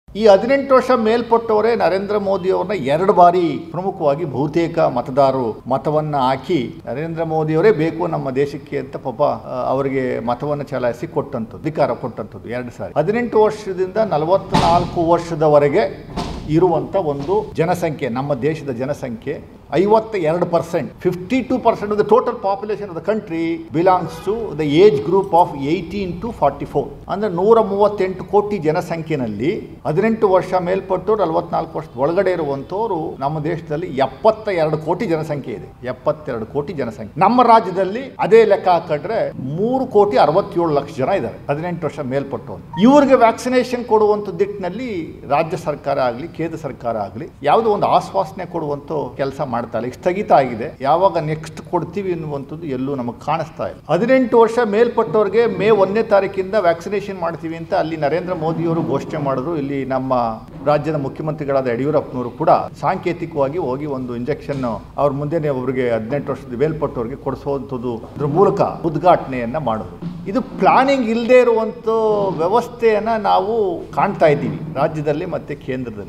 18 ವರ್ಷ ಮೇಲ್ಪಟ್ಟವರಿಂದಲೇ ಮೋದಿ ಪ್ರಧಾನಿಯಾಗಿದ್ದು, ಈಗ ಅವರಿಗೆ ಲಸಿಕೆ ಸಿಕ್ತಿಲ್ಲ-ಲಕ್ಷ್ಮಣ್
18 ವರ್ಷ ಮೇಲ್ಪಟ್ಟವರಿಂದಲೇ ಮೋದಿ ಪ್ರಧಾನಿಯಾಗಿದ್ದು, ಈಗ ಅವರಿಗೆ ಲಸಿಕೆ ಸಿಕ್ತಿಲ್ಲ-ಲಕ್ಷ್ಮಣ್